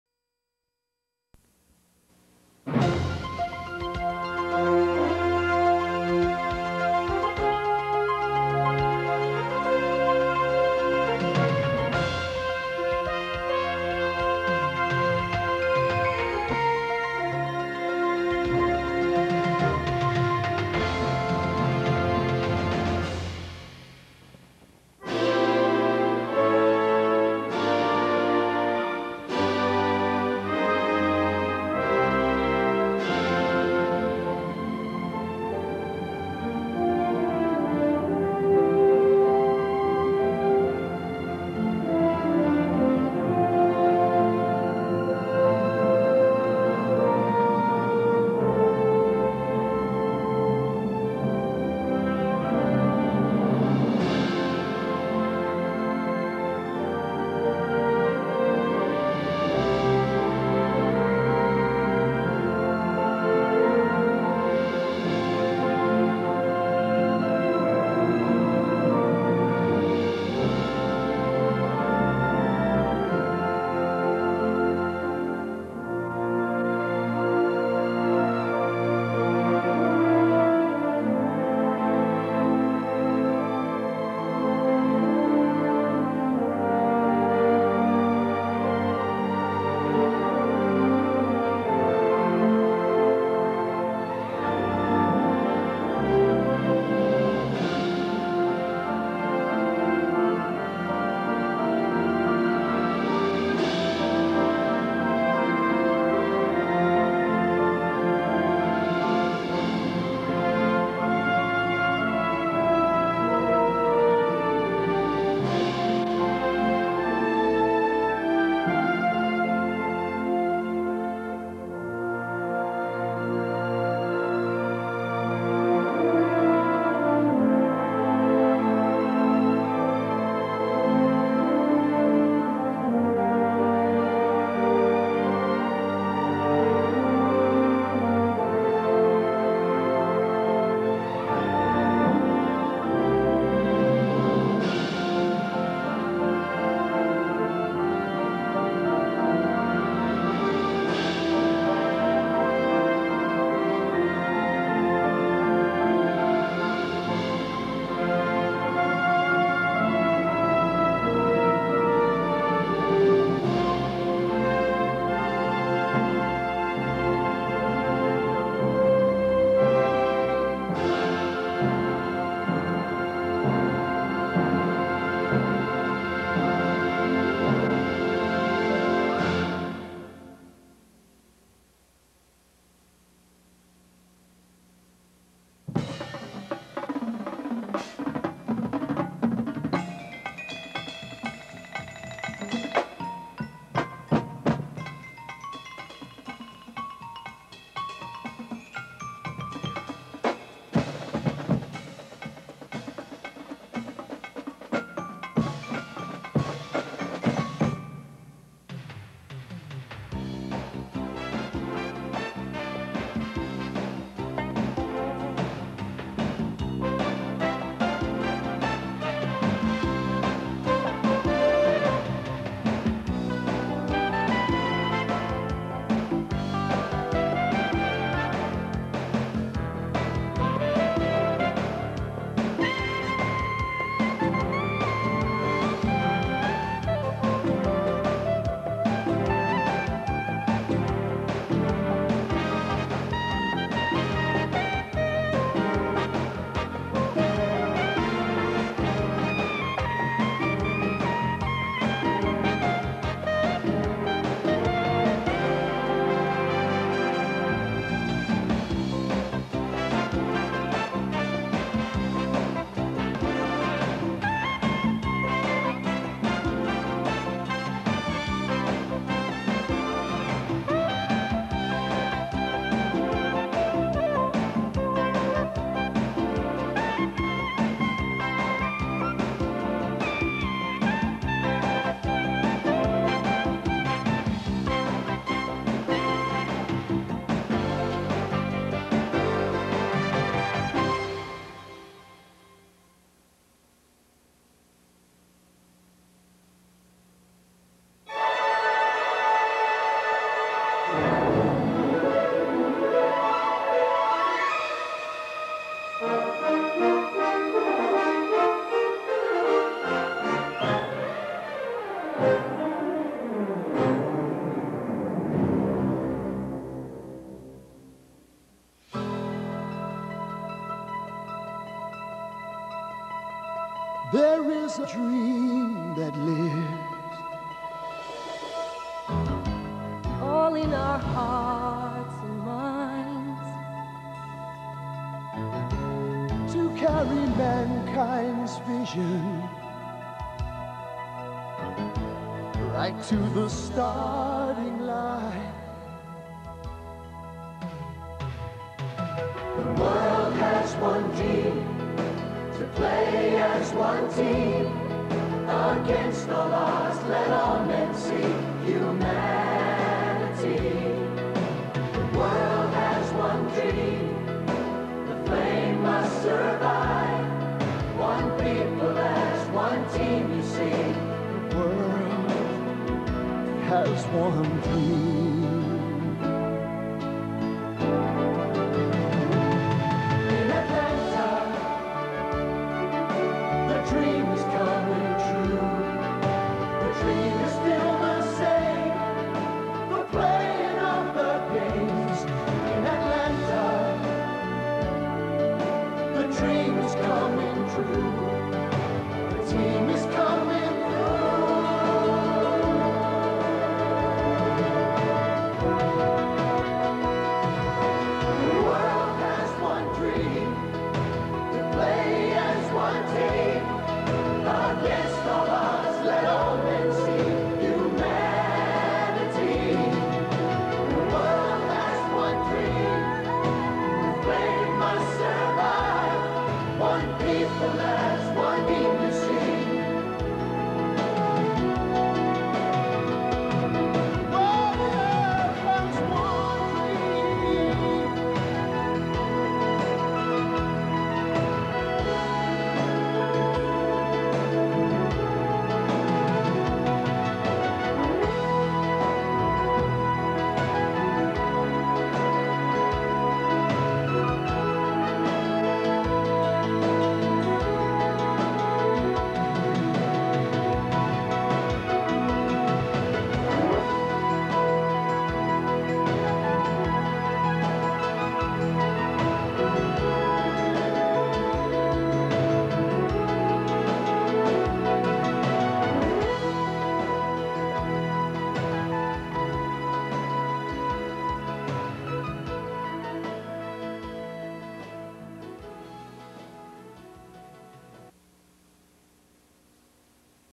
1996 Atlanta Olympic Bid Video
Part of the official bid presentation for the 1996 Centennial Olympic Games.
Music beds only. live narration during presentation to the IOC.